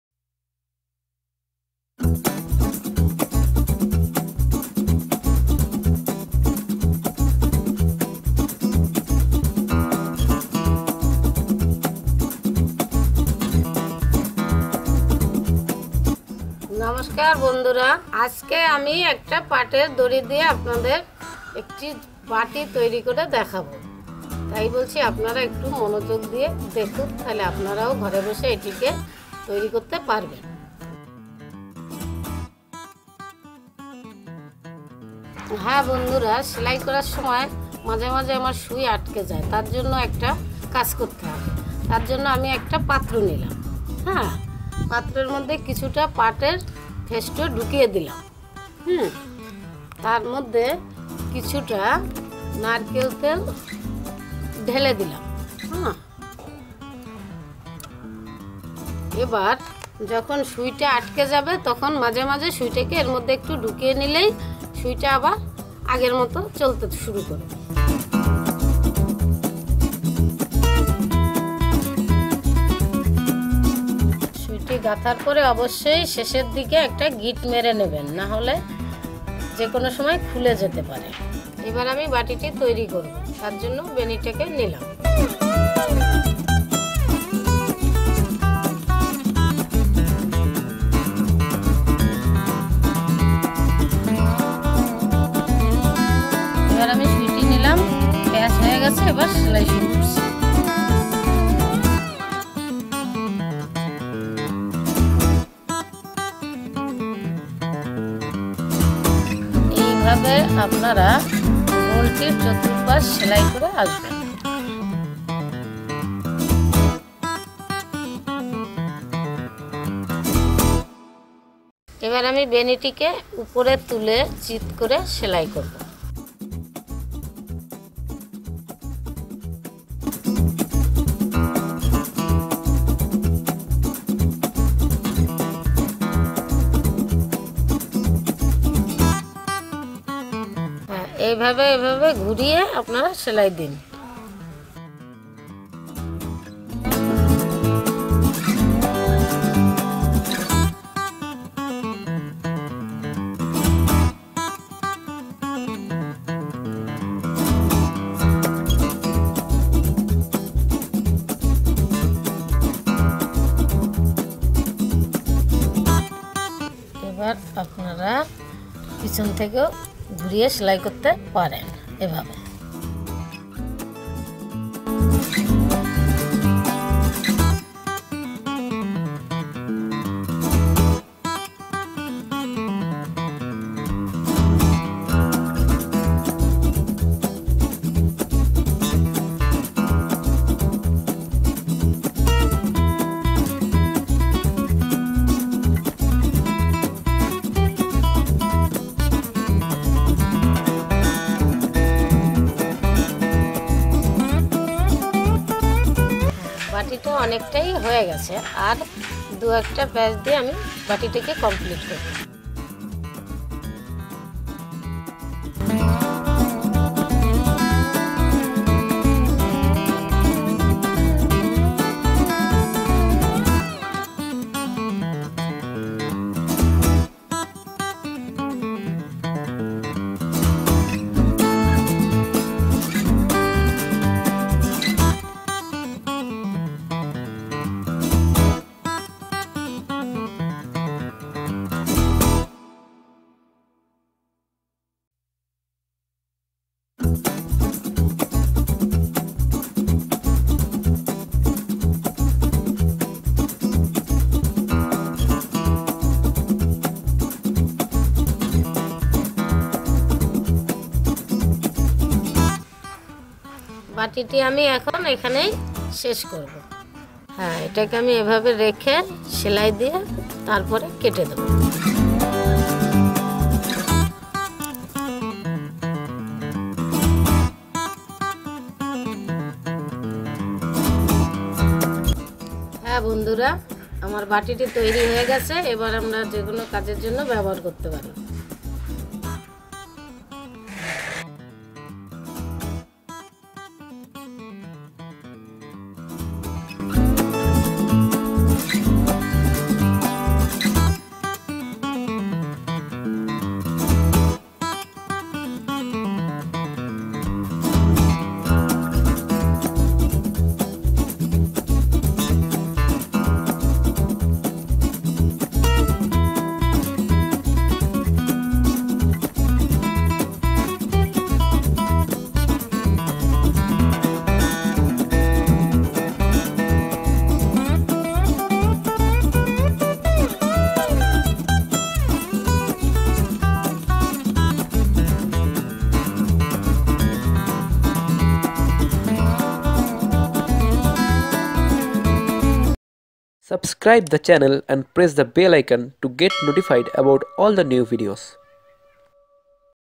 Very Useful and Attractive Jute BASKET DIY - Its easy
Hi all my dear friends, in this video I Make an attractive jute basket using jute rope, you can also make the same designed basket or bowl at your home very easily.